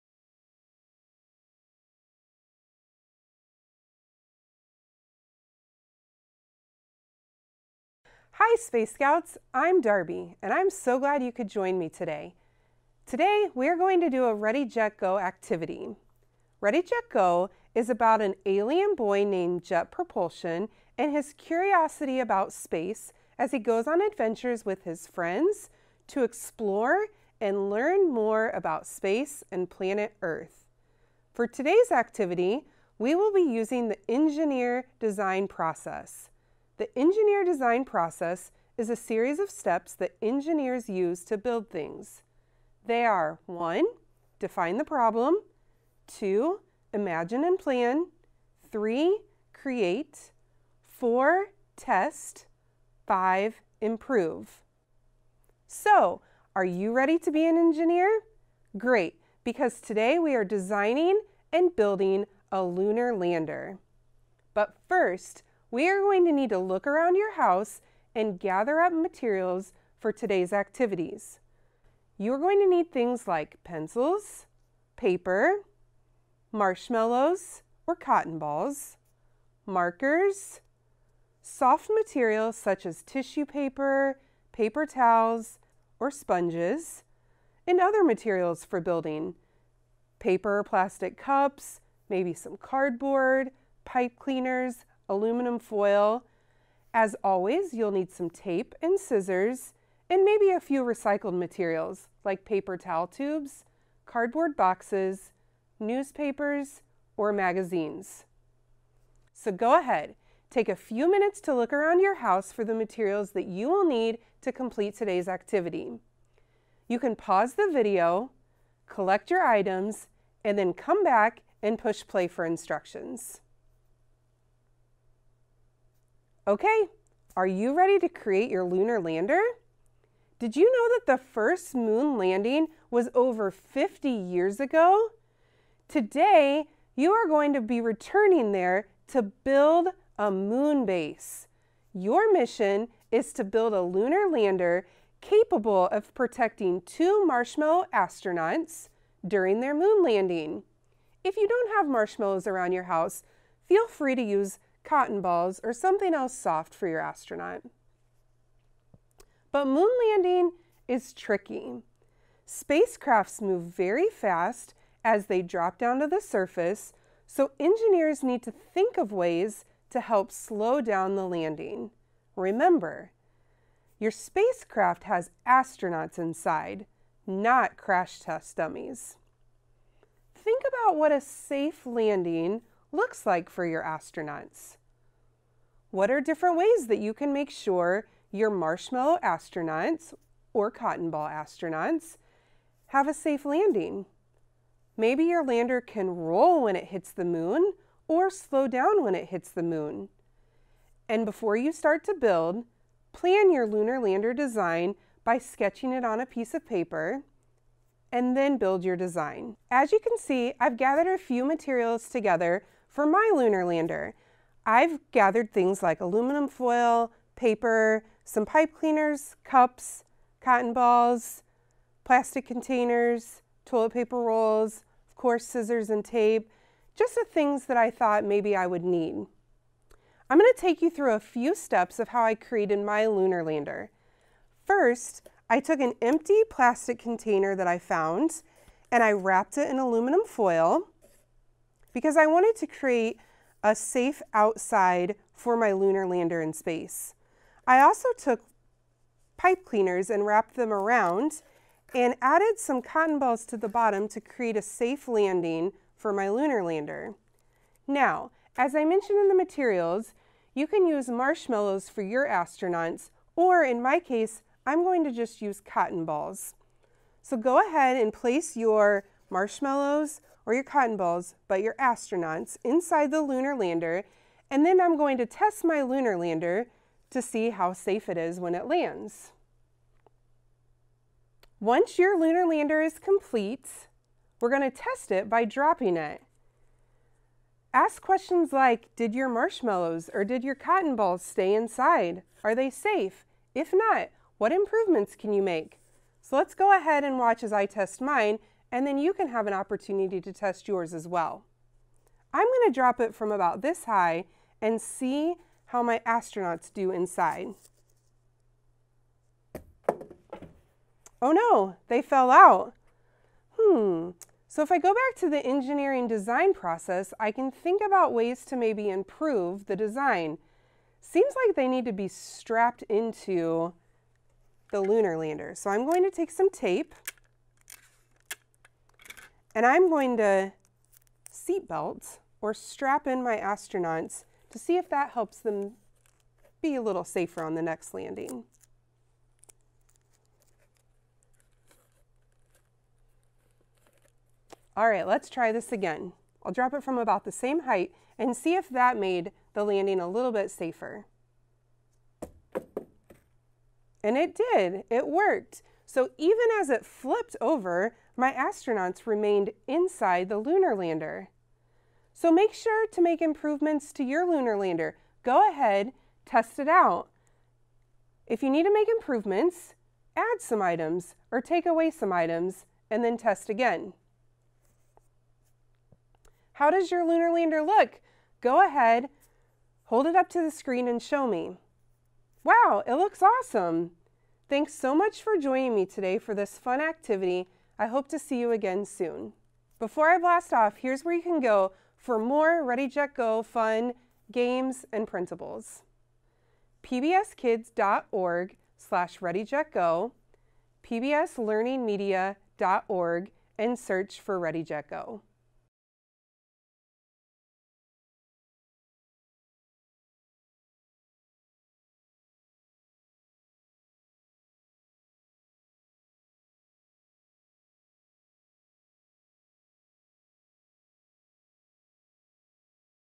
Let's Make a Lunar Lander
Jet and his friends need your engineering skills to help build a Lunar Lander to keep the astronauts safe when they land on the moon!

Host - Darbi Strahle
Director - Sarah Kreps
Producer - Michelle Kinsey
Station - Ball State PBS